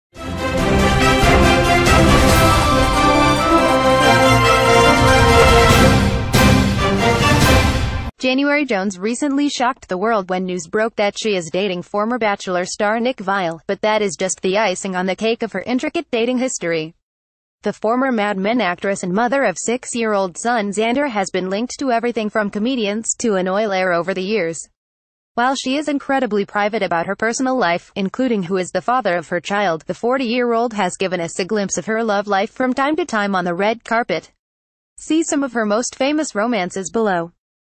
Comedian, actor and Bachelor: 11 men who were associated with January Jones
January Jones recently shocked the world when news broke that she is dating former Bachelor star Nick Viall, but that is just the icing on the cake of her intricate dating history. The former Mad Men ...